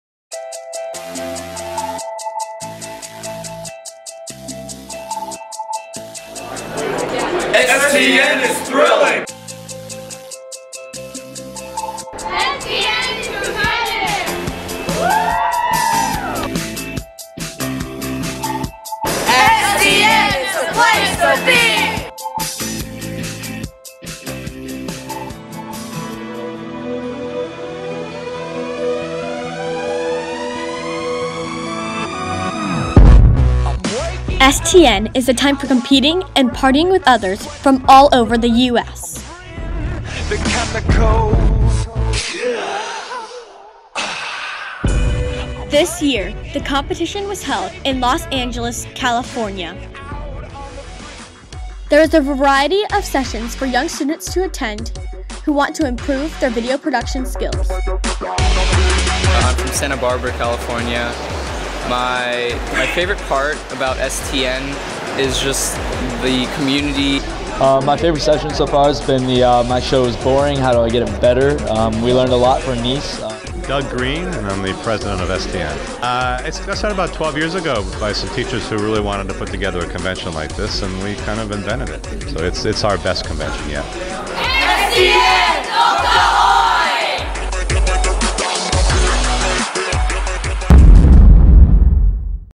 STN Story 2013 Maitland Middle School
A video we made for US Education. We were the only middle school out of 5 schools to get $500! I edited this with Maddie and Alexis Kidd.